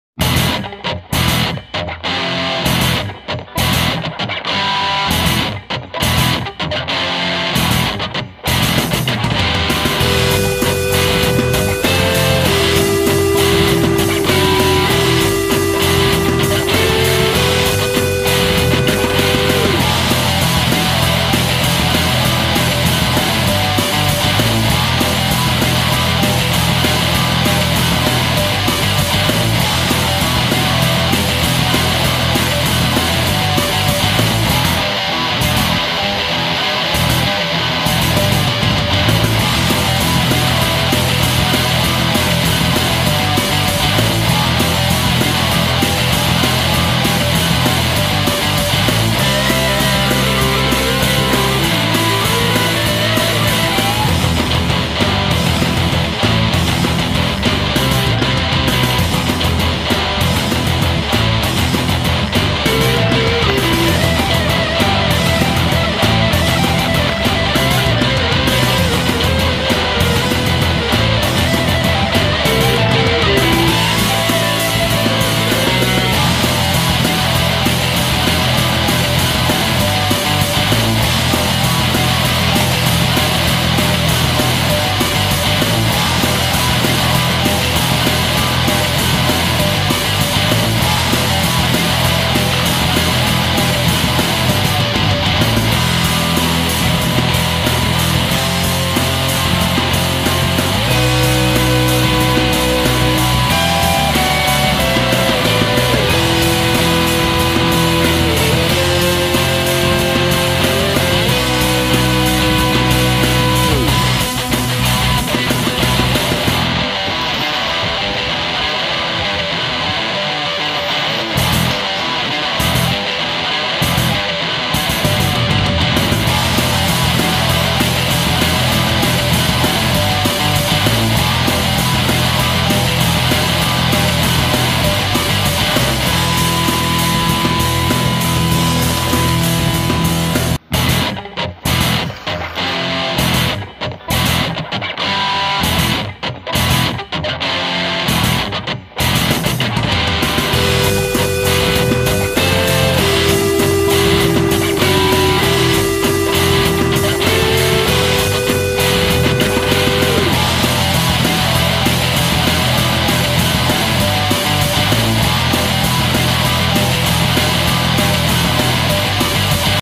WGB 2021 COLOUR CODED RACE R1 M1 AMC REBEL MACHINE V NITRO V 52 CHEVY
WGB RACE 2021 IS A GROUP COLOUR OF RACING CAR DIVIDE INTO WHITE GREY AND BLACK.BEST OF 3 LAP AND HAVE WIN THE MOST AUTOMATIC QUALIFY INTO NEXT STAGE